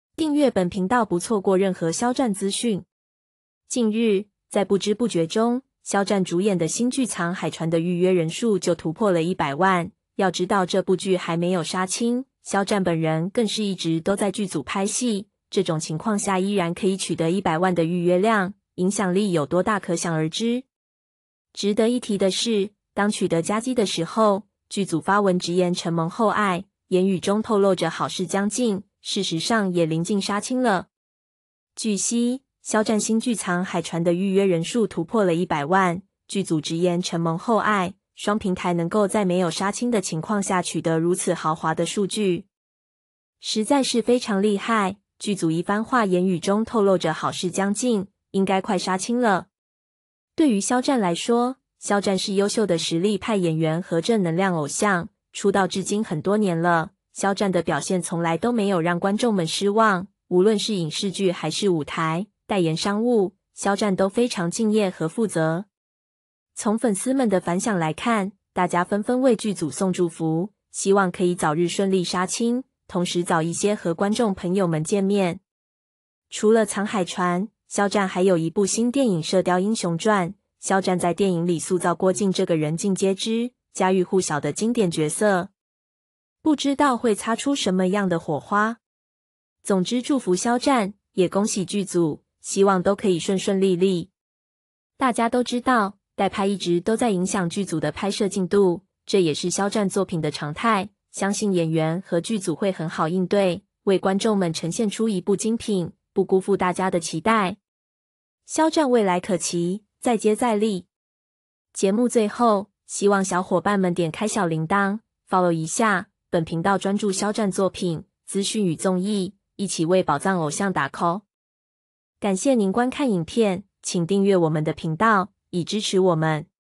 肖戰《藏海傳》破100萬預約量！劇組直言承蒙厚愛，透露好事將近
更新：频道主的个人感受：肖战真的有魅力在身上，我看见了你作为一个普通人，走向世界，绽放你的魅力，真的太励志了，太感动了，说句粗话，你真的有两把刷子在身上，我真的佩服你。